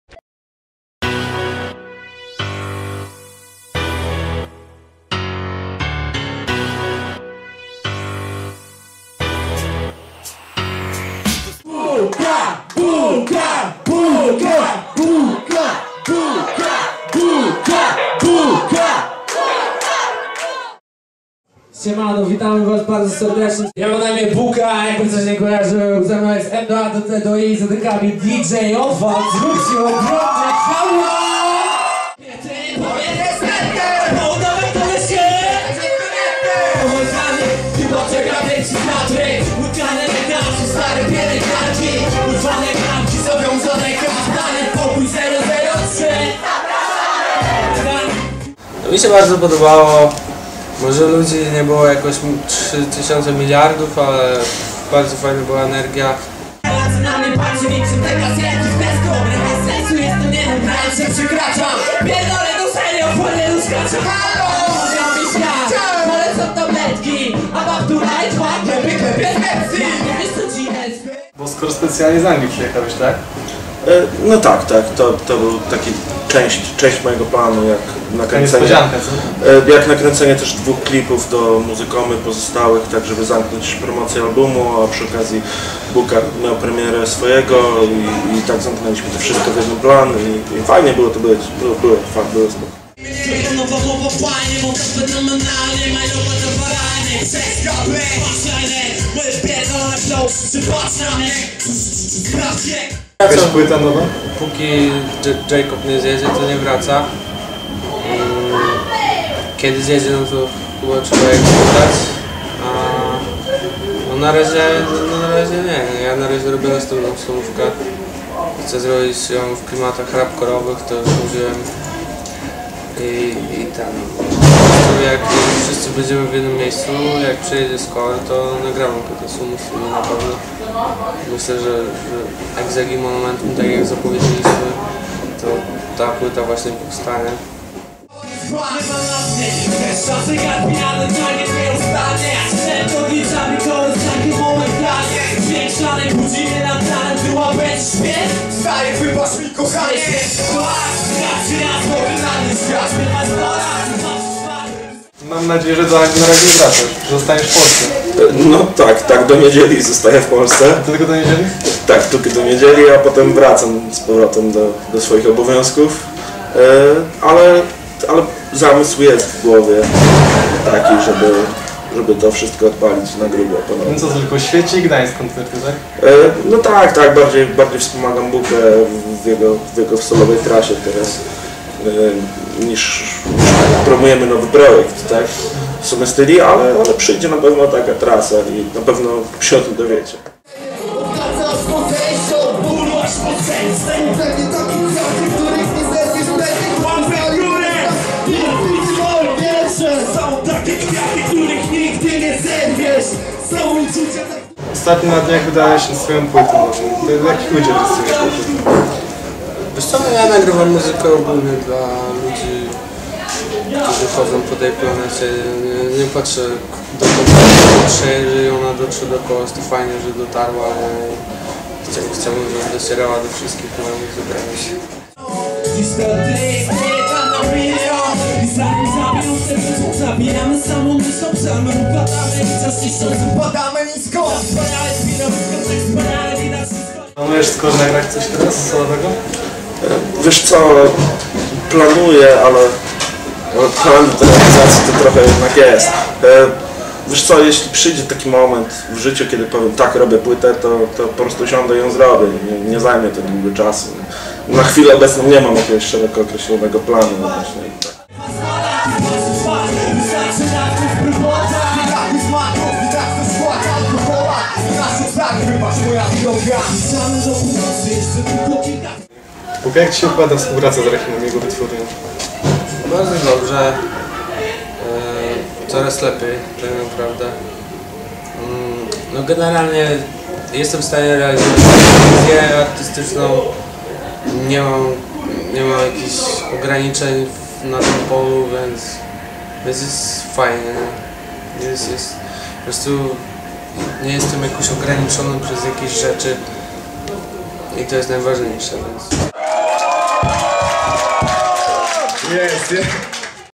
Buka, Skor, SumaStyli | WYWIAD, RELACJA | ŚWIECIE
Koncert Buka, Skor, SumaStyli odbył się w Kręgielni Bowlingplanet w Świeciu.